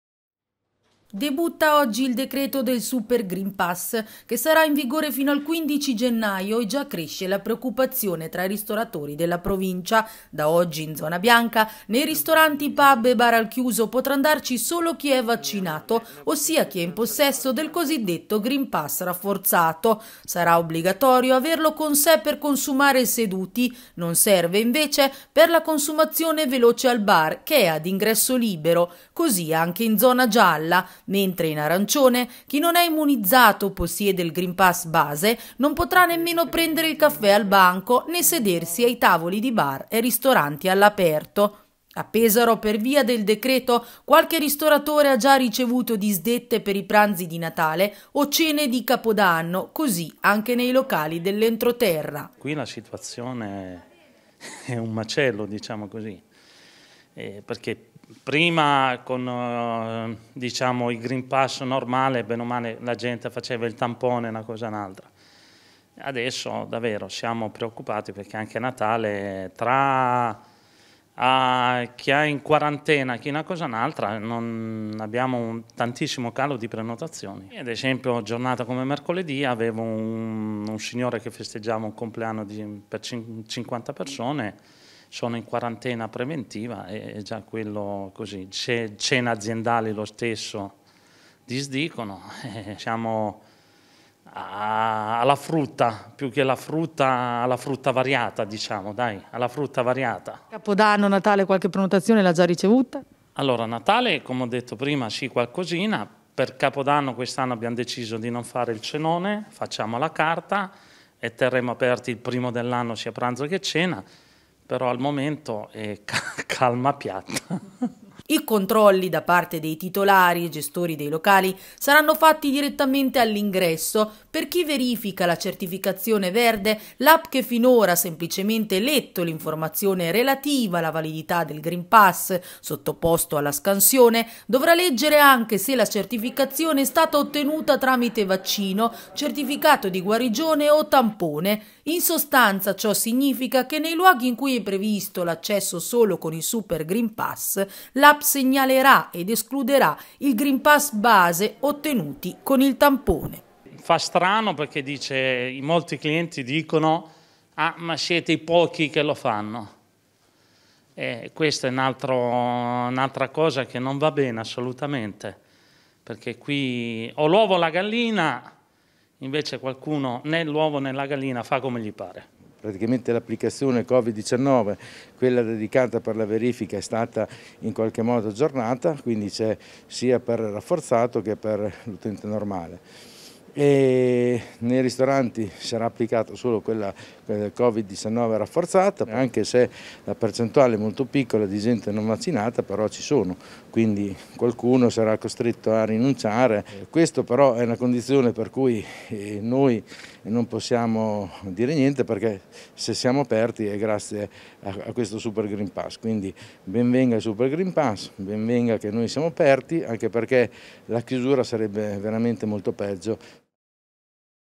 Super Green Pass, non vaccinati esclusi dai ristoranti
Da lunedì entra in vigore il decreto del Super Green Pass, che sarà valido fino al 15 gennaio. Chi non sarà in possesso della certificazione verde rafforzata, non potrà più sedersi né consumare ai tavoli di bar e ristoranti al chiuso. Qualche ristoratore della Provincia ha già ricevuto disdette e c'è in generale, un po' di preoccupazione per un possibile calo delle prenotazioni, da qui al prossimo mese.